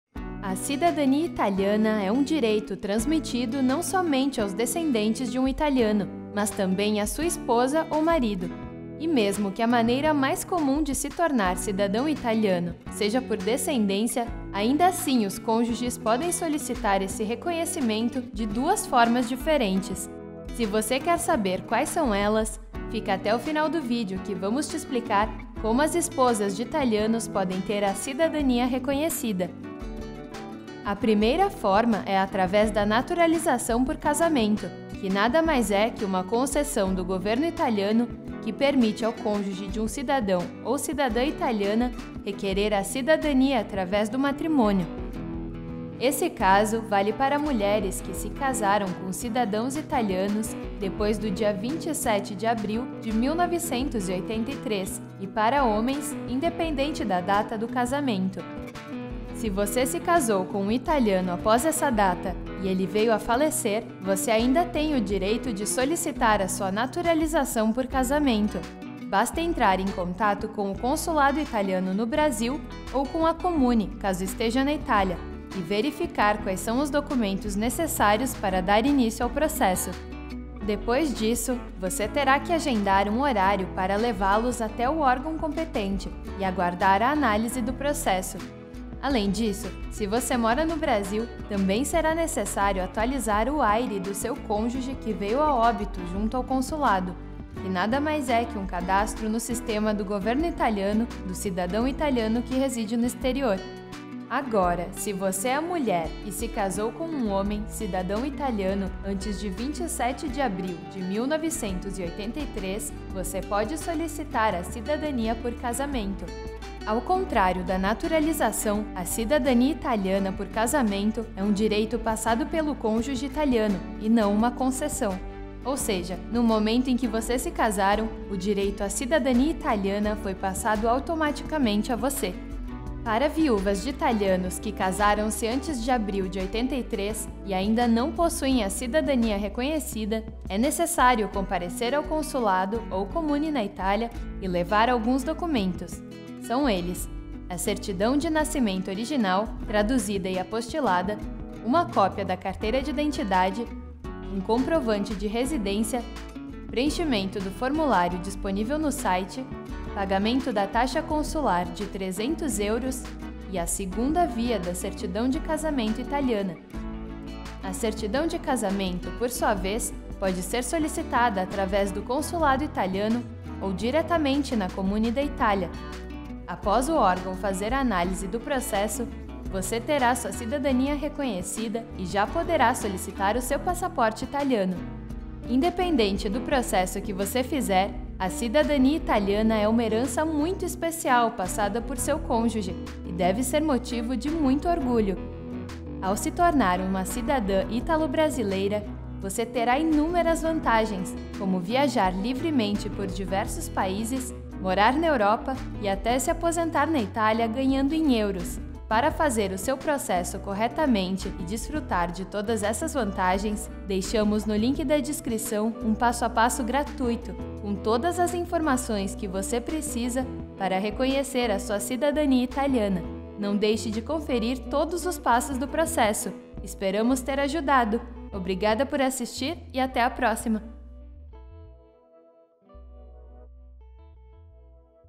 CIDADANIA ITALIANA PARA VIÚVAS DE ITALIANOS 🤔
CIDADANIA ITALIANA PARA VIÚVAS DE ITALIANOS SERÁ QUE TEM DIREITO? No Vídeo De Hoje Iremos Responder Uma Dúvida Muito Frequente Entre Pessoas Interessadas Em Obter a Cidadania Italiana, Confira! 😄

Acesse mais benefícios como empregos, bolsas-de-estudos, morar na Itália sabendo falar italiano! Veja mais detalhes aqui: